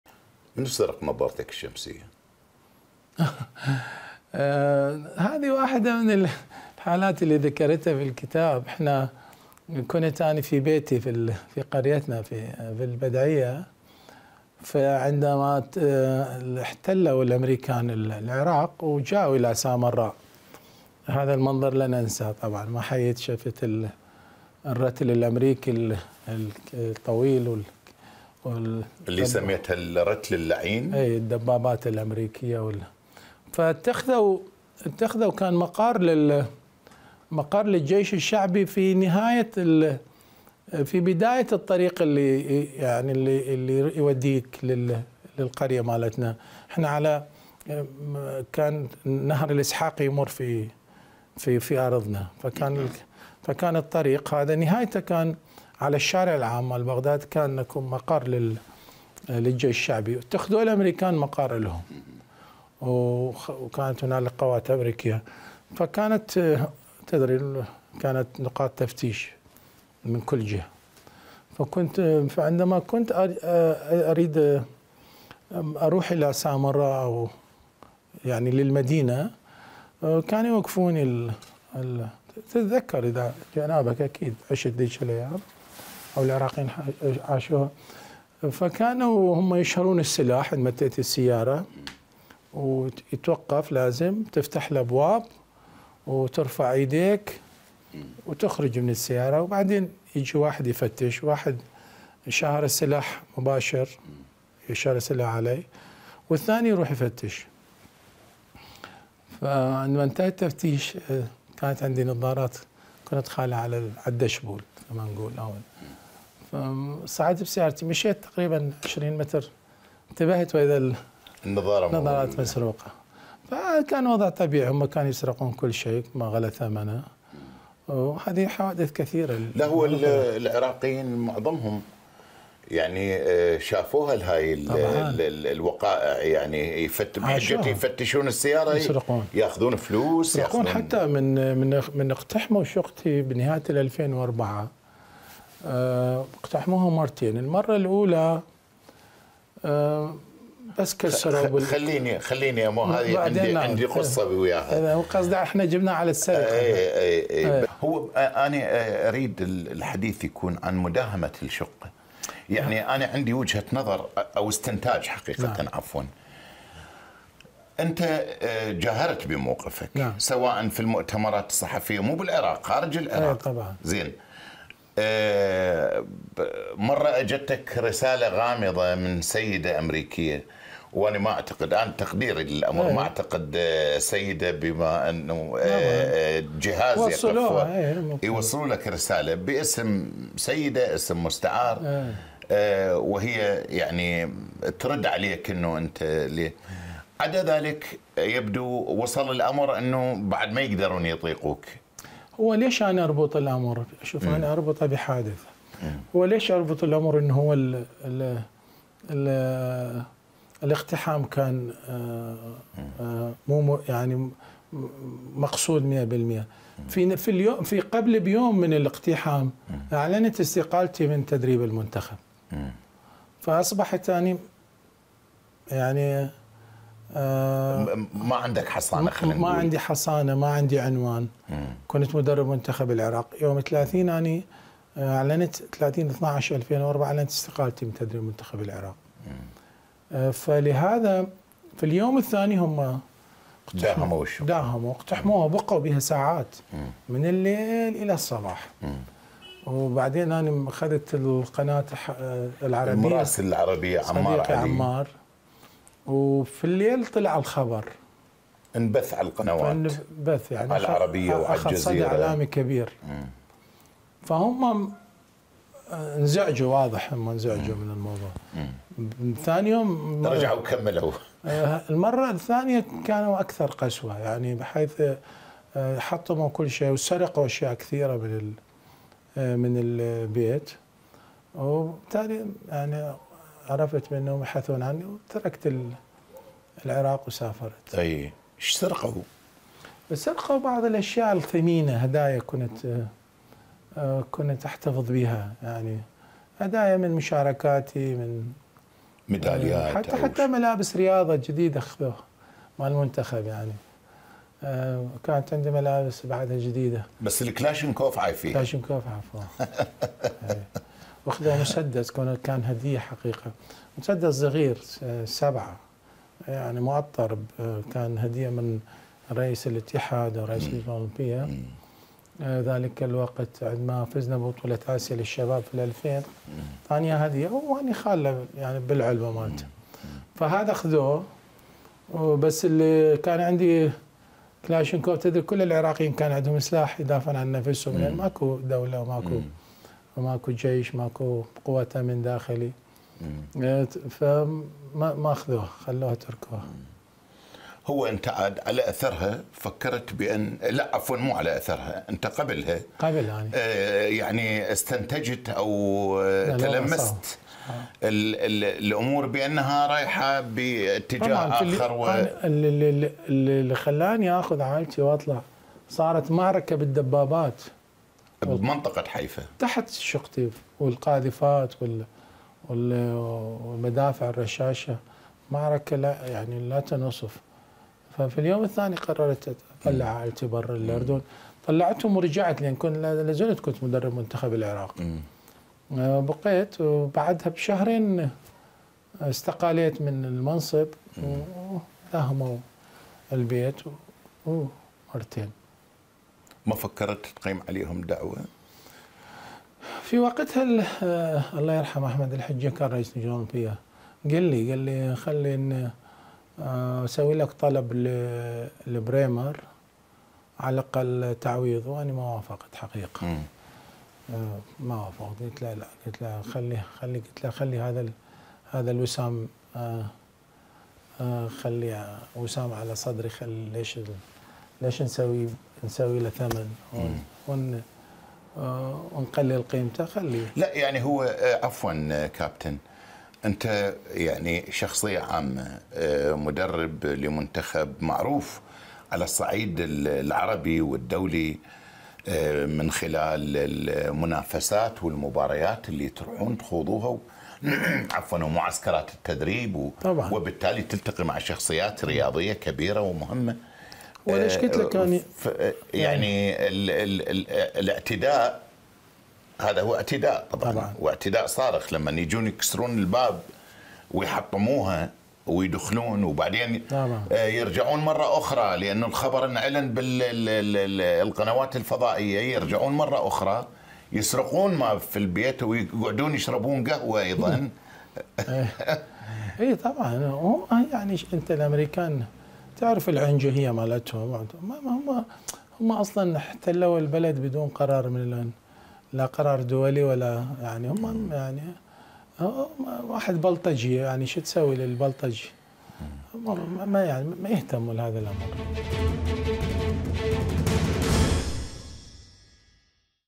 عدنان حمد و تفاصيل مثيرة حول اقتحام منزله وسرقة سيارته؟
عدنان حمد وتفاصيل اقتحام منزله وسرقة سيارته؟

موقع قناة التغيير الفضائية الالكتروني